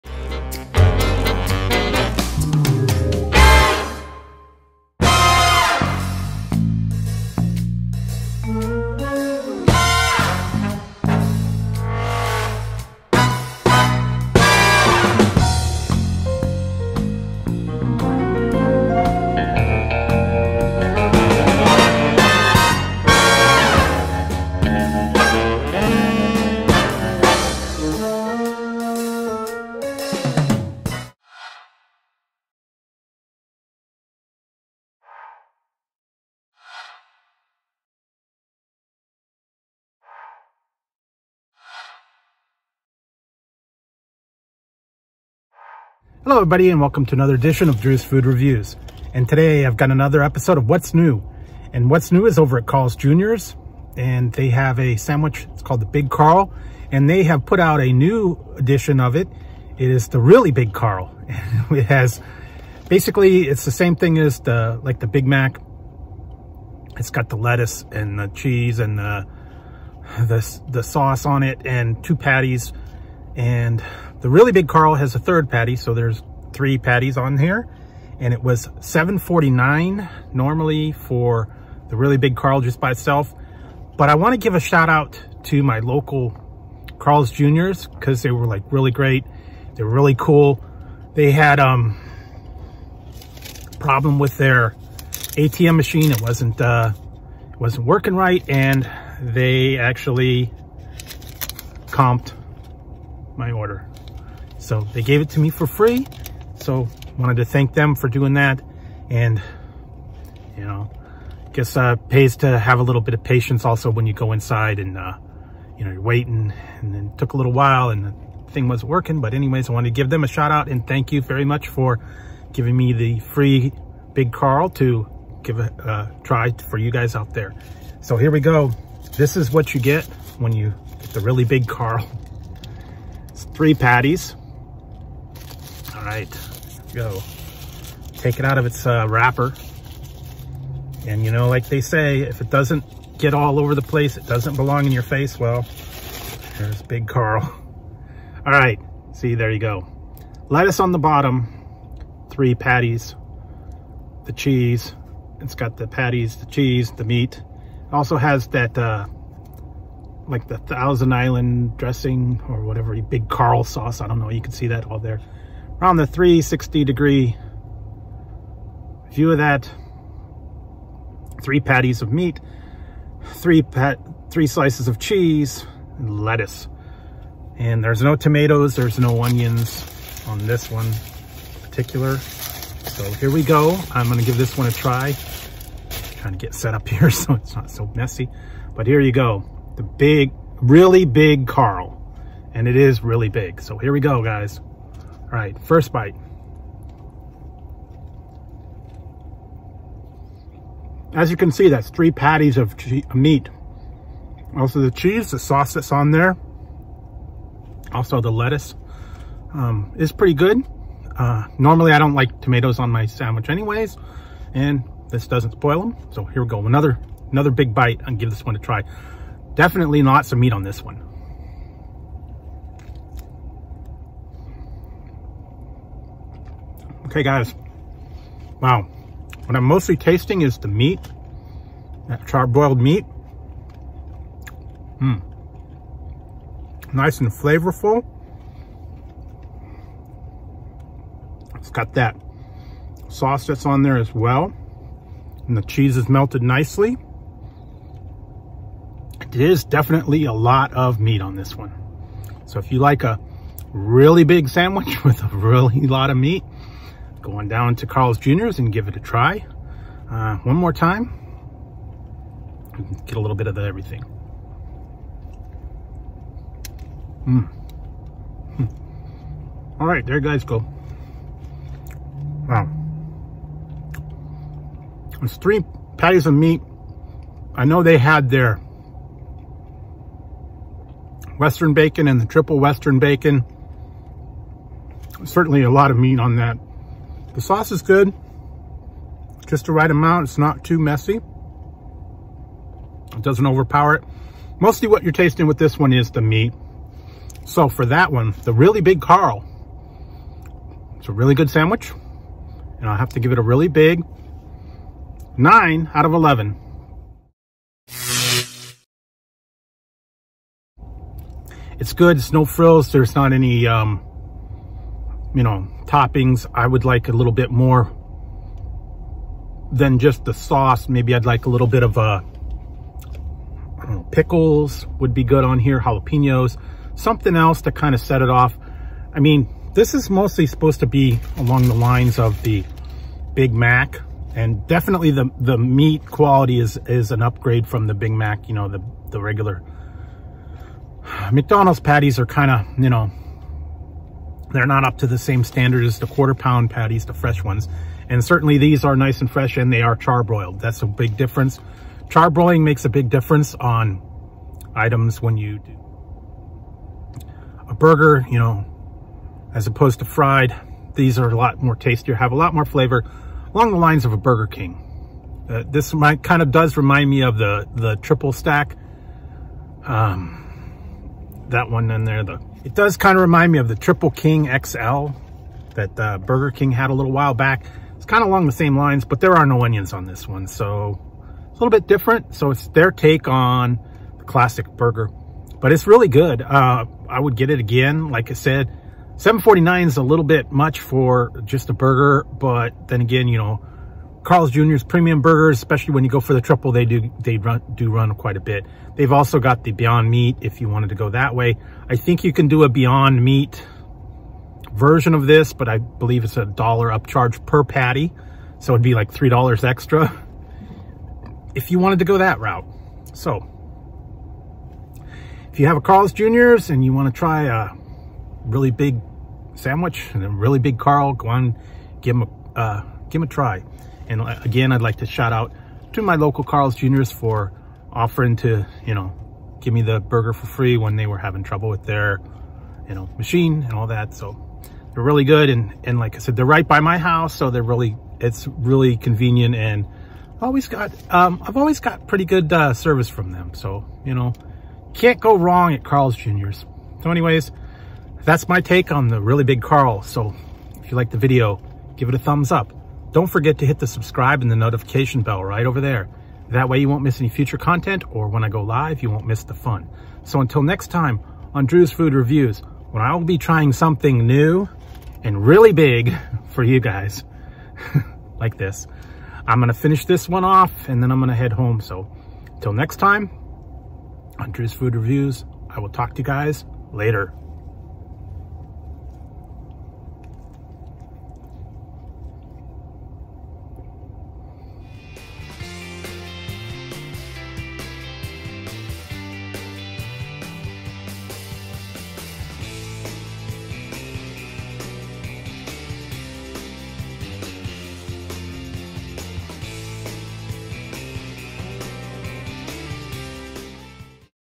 What’s New : Carl’s Jr. The Really Big Carl ⭐️
Today on Whats New I’m trying the new Really Big Carl from Carl’s Jr. let’s see if this one has really big taste or not also thanks to my local Carl’s Jr. for the comp meal.